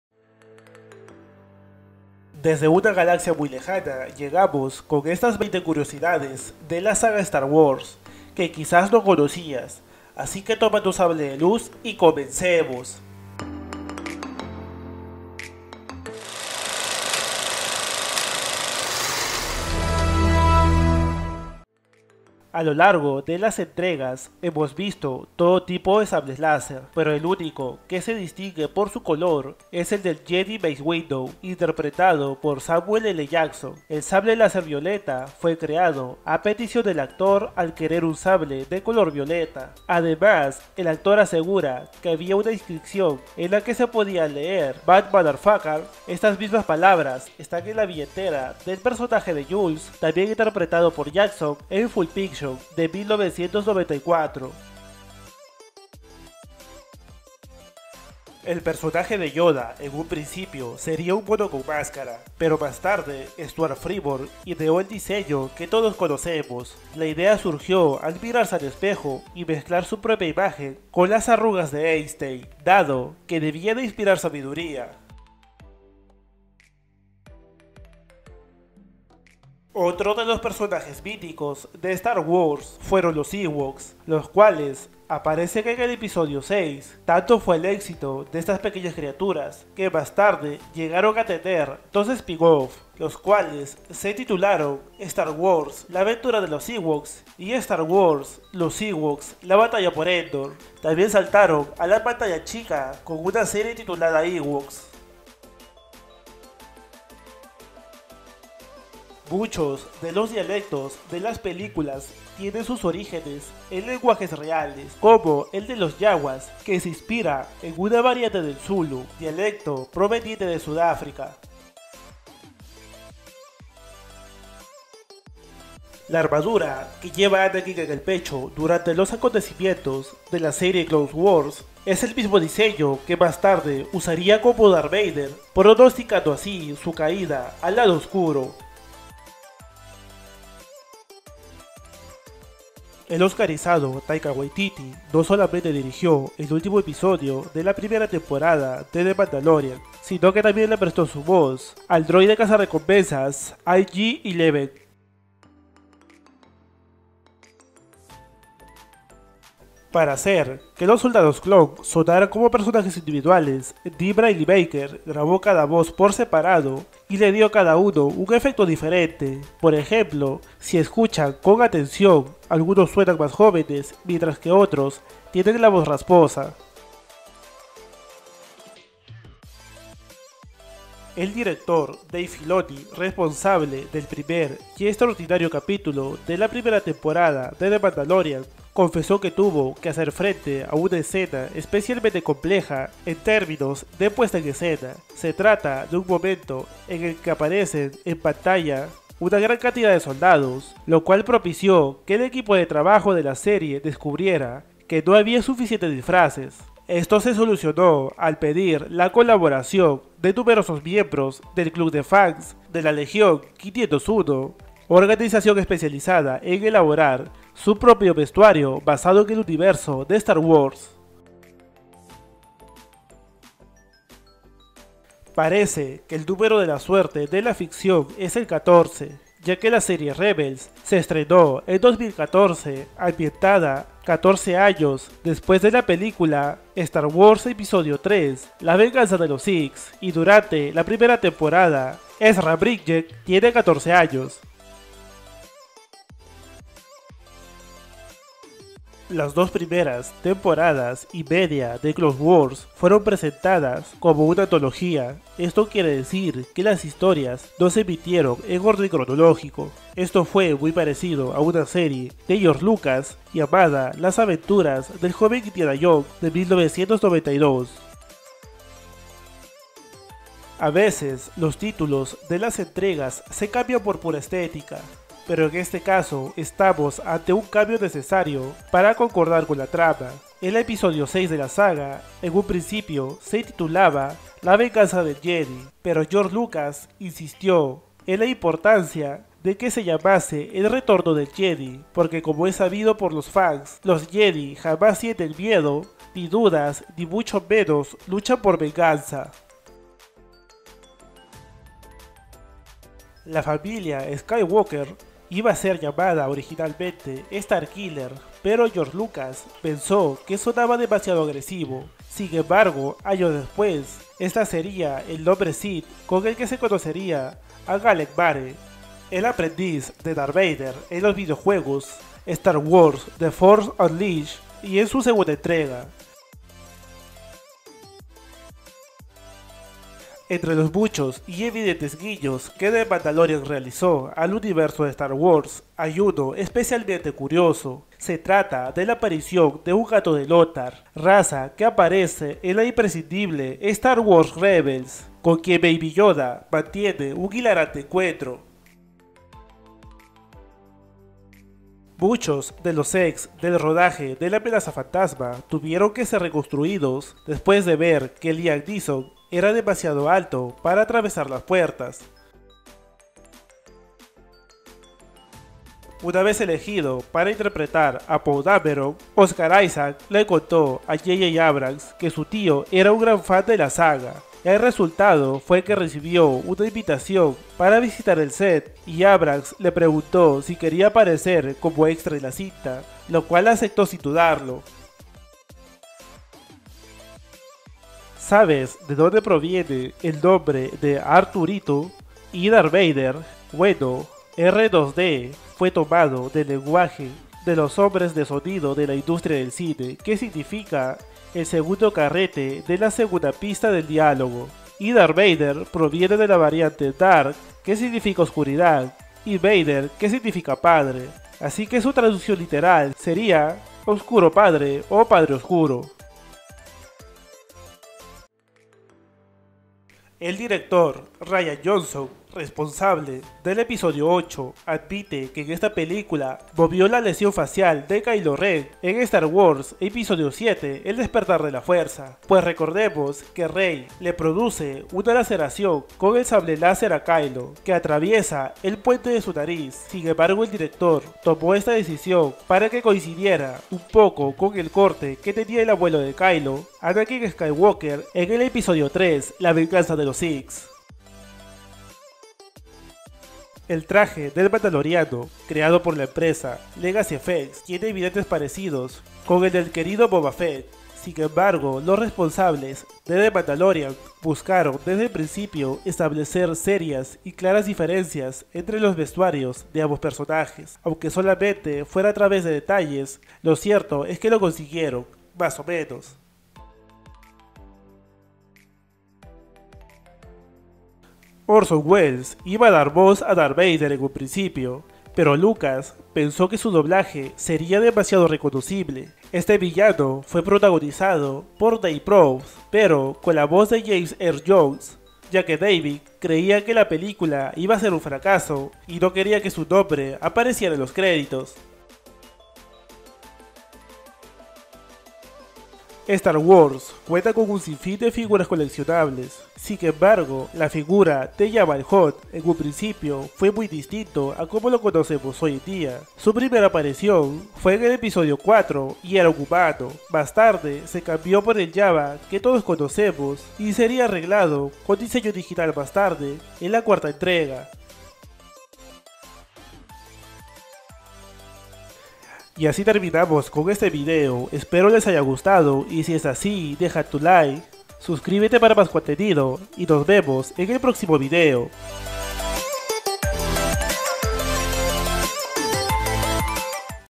20 CURIOSIDADES DE STAR WARS / Jabba el Hutt no siempre fue como lo conocemos
Star wars es todo un clásico del cine y la cultura pop que sigue fascinando a todo el mundo. En este video te contare algunas curiosidades que debes saber sobre esta increíble saga galáctica.